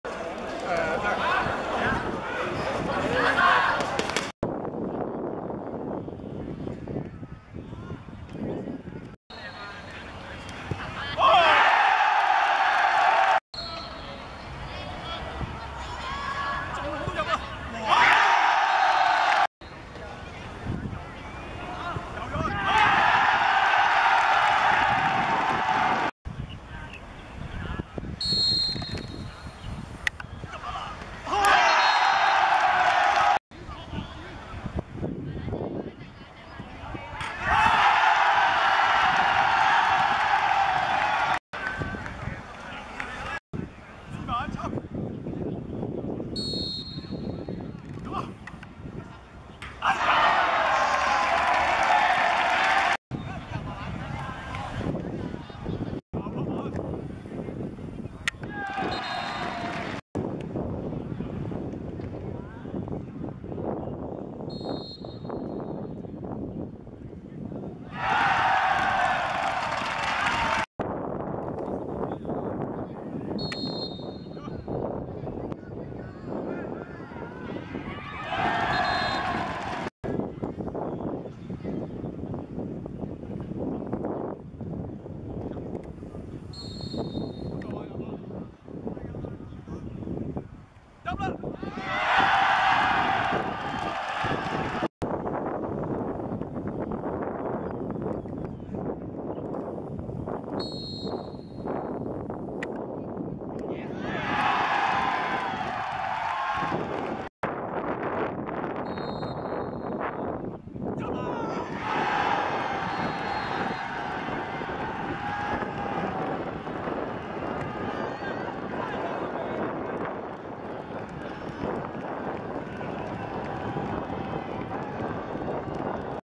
聖若瑟書院vs拔萃(2014.4.11.學界D1足球丙組決賽)~峰廻路轉決勝點球篇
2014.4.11.蒲崗村仿真草塲 決勝點球賽果 : 聖若瑟書院 3:2 拔萃